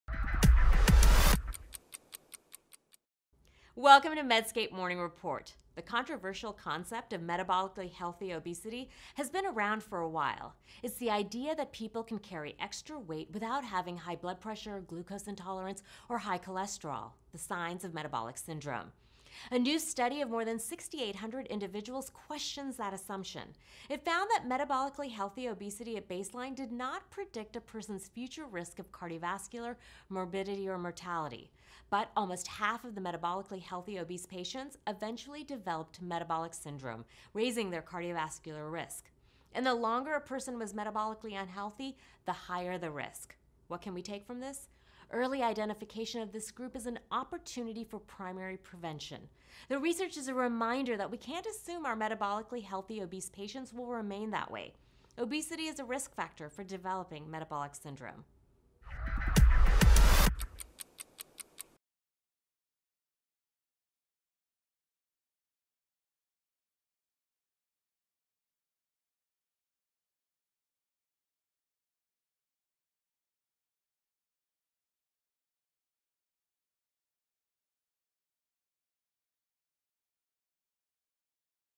Can Obesity Ever Be Healthy? | Morning Report
A new study of more than 6800 individuals questions that assumption. It found that metabolically healthy obesity at baseline did not predict a person's future risk for cardiovascular morbidity or mortality. Almost half of the metabolically healthy obese patients eventually developed metabolic syndrome, raising their cardiovascular risk. And the longer a person was metabolically unhealthy, the higher the risk.